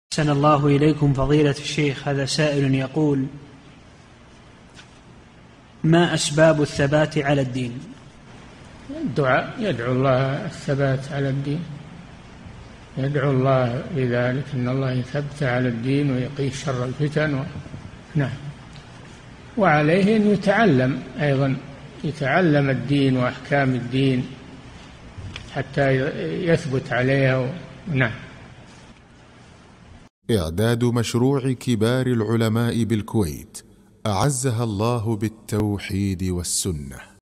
[2617 -3022] ما أسباب الثبات على الدين؟ - الشيخ صالح الفوزان
-"[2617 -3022] السؤال: ما أسباب الثبات على الدين؟
الجواب: الجَوَابُ: الدعاء يدعو الله الثبات على الدين، يدعو الله بذلك أن الله يثبته على الدين ويقيه شر الفتن. وعليه أن يتعلم أيضا، يتعلم الدين وأحكام الدين، حتى يثبت عليها.
للاشتراك في قناة الشيخ صالح الفوزان

للاستفسار والإقتراحات والدعم
رئيس الفريق
لمشروع كبار العلماء